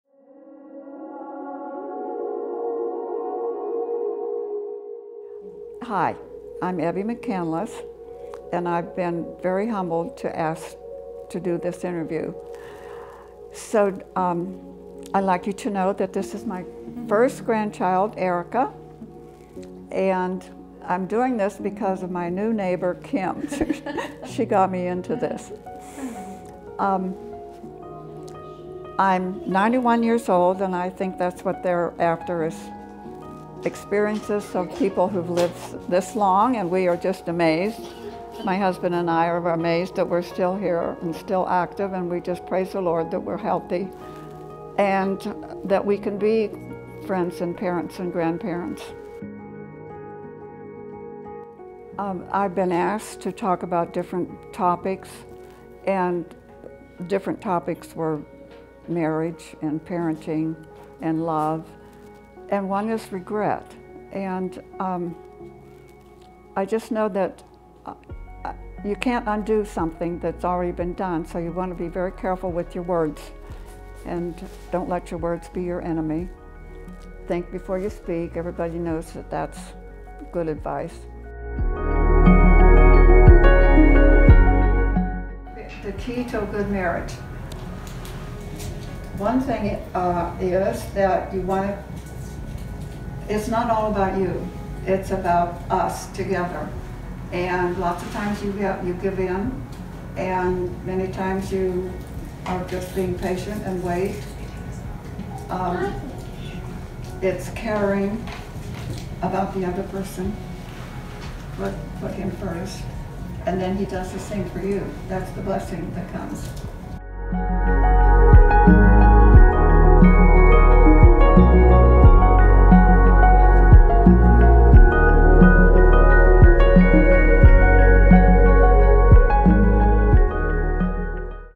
Evie McCandless - 9 Things I've Learned in Nine Decades
This video is a behind the scenes look at Evie McCandless' photoshoot for her article featuring the 9 things she has learned in her 91 years.